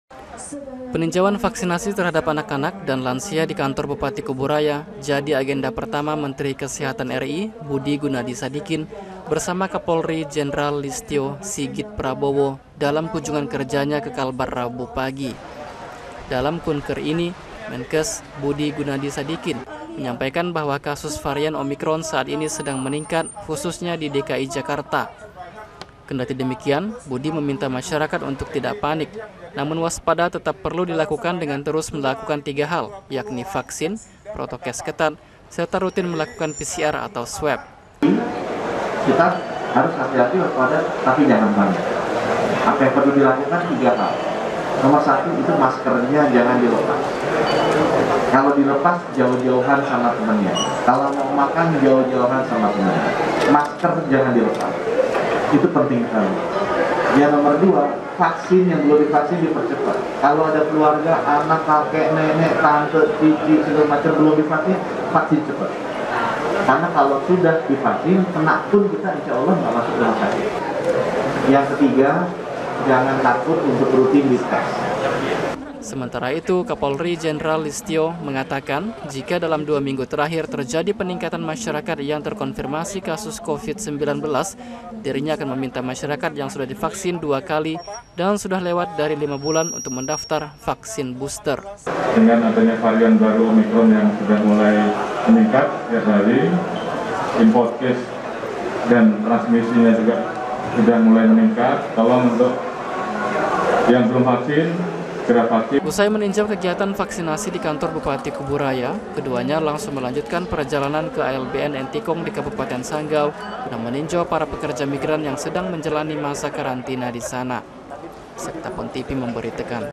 Menkes Minta Masyarakat tak Panik Varian Omicron
Menkes Budi Gunadi Sadikin bersama Kapolri Jenderal Listyo Sigit Prabowo, berkunjung ke Kalbar Rabu pagi. Selain menekankan percepatan vaksinasi, Menkes Budi dalam kunker itu juga menyampaikan pesan agar masyarakat jangan panik varian omicron Covid-19, namun waspada tetap harus dilakukan